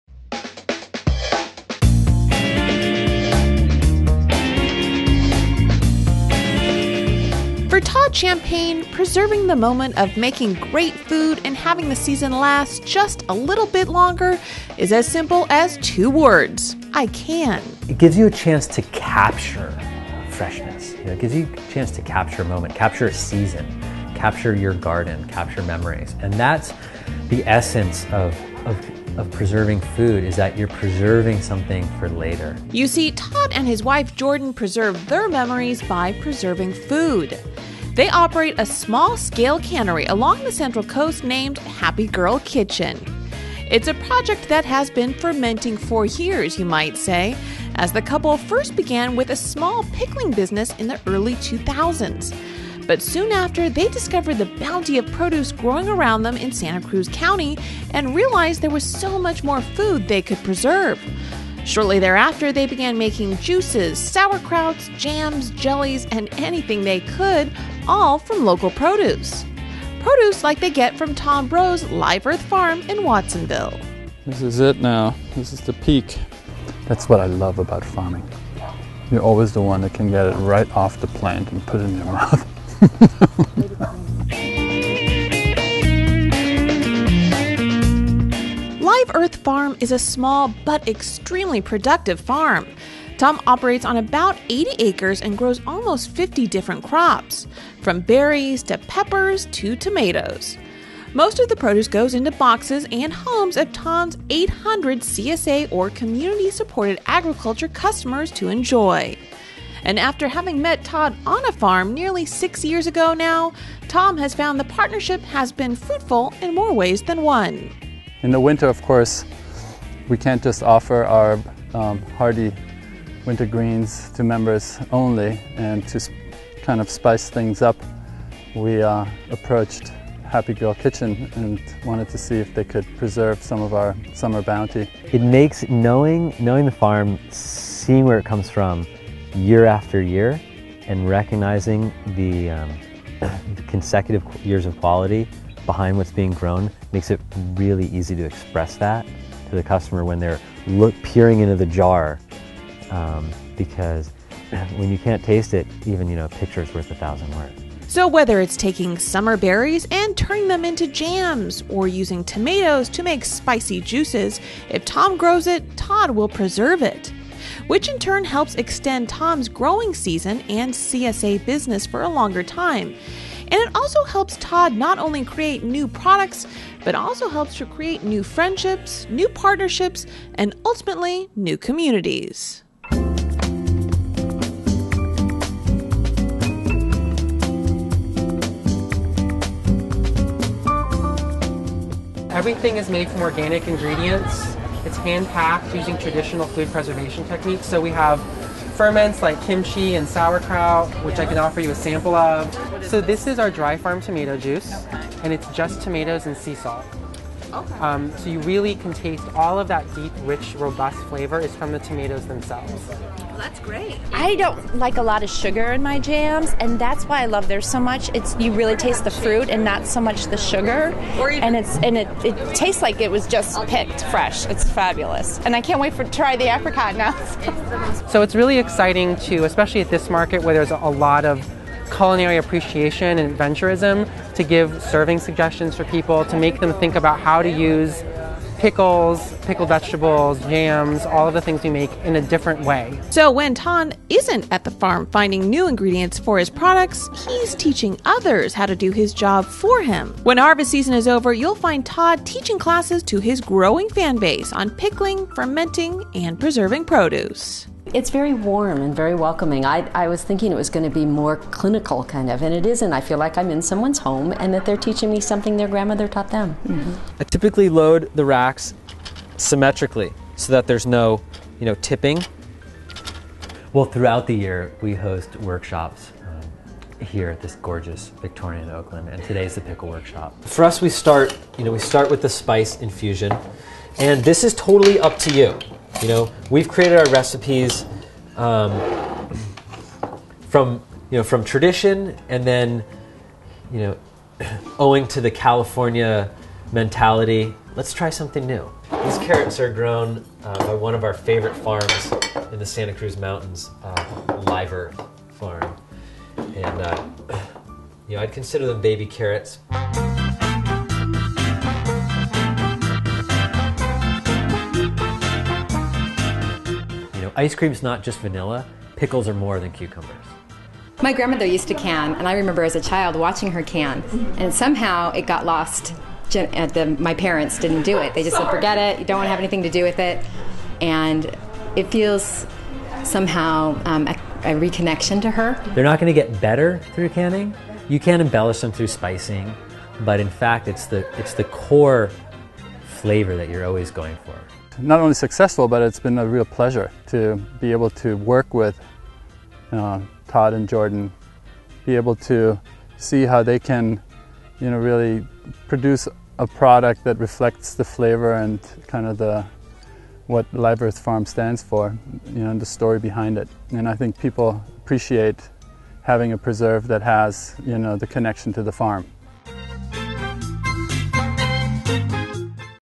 Capturing time in a bottle
Artisans and a farmer help others rediscover the lost art of canning.